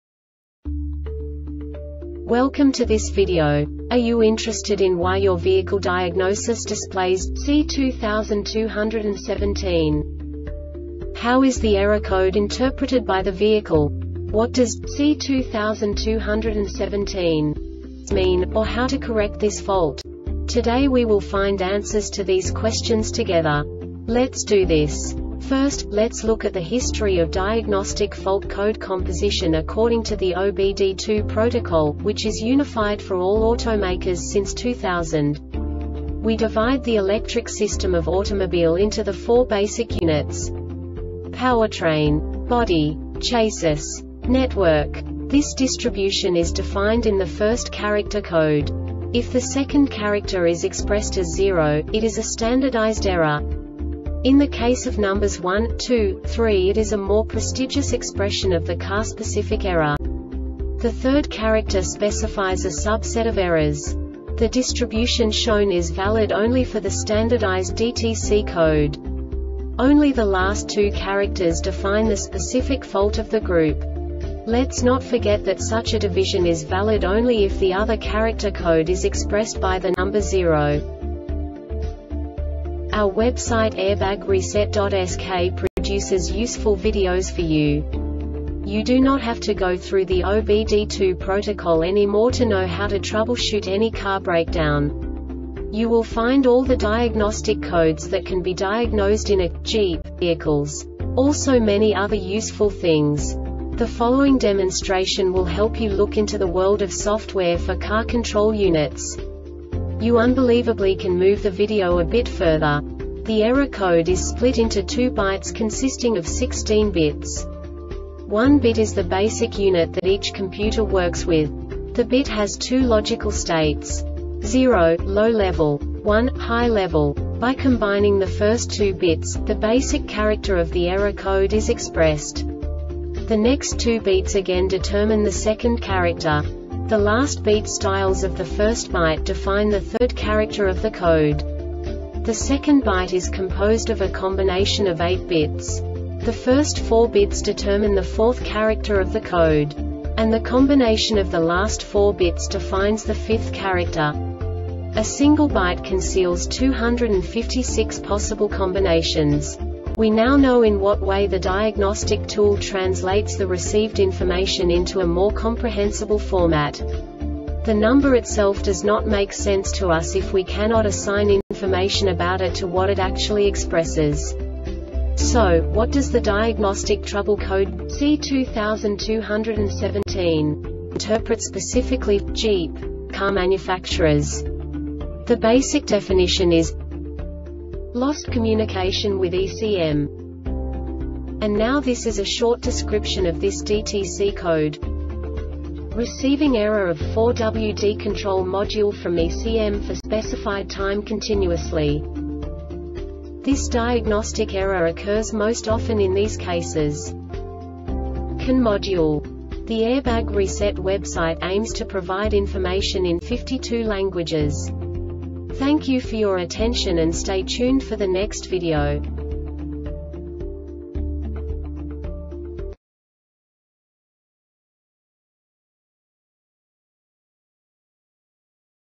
DTC Jeep C2217 Short Explanation
Contents:
0:21 Basic DTC analysis according to OBD2 protocol standard.
1:48 Insight into programming
2:58 A diagnostically understandable description of the Diagnostic Trouble Code
3:05 Basic error definition

Make
Jeep
Code
C2217
Definition
Lost Communication With ECM
Description
Receiving error of 4WD control module from ECM for specified time continuously
Cause
CANModule